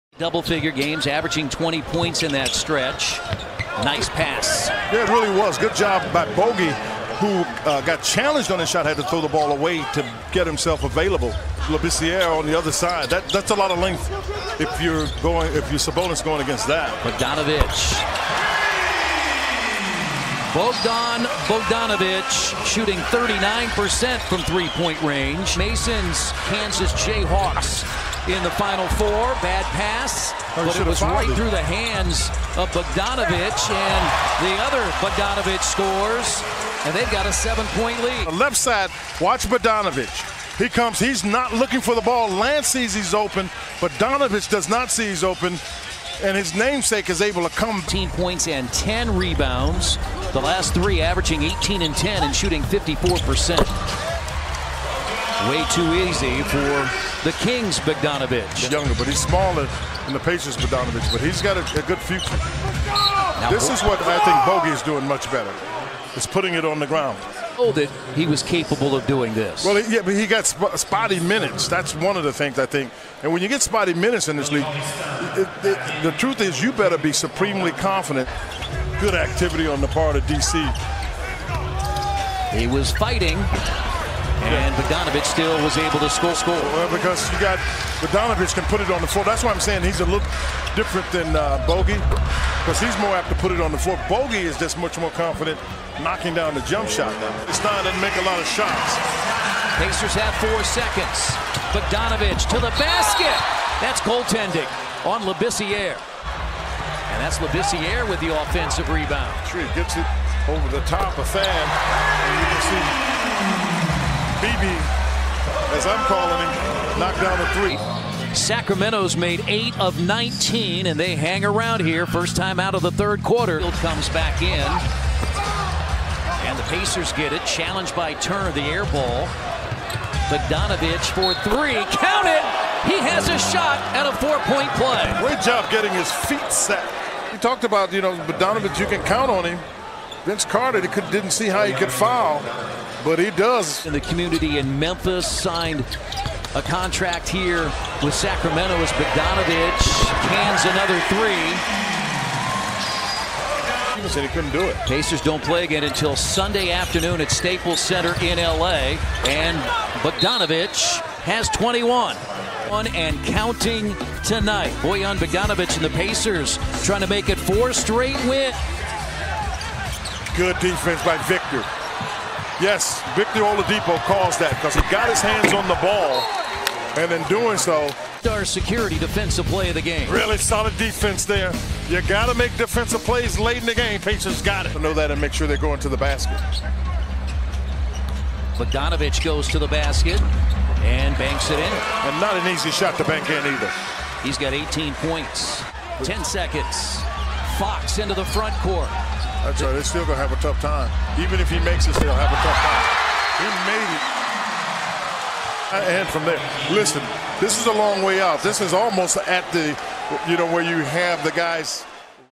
Batalla de BOGDANOVIC´S BOJAN VS BOGDAN CROACIA VS SERBIA
Gran partido entre jugadores Europeos que ahora juegan en la mejor liga del mundo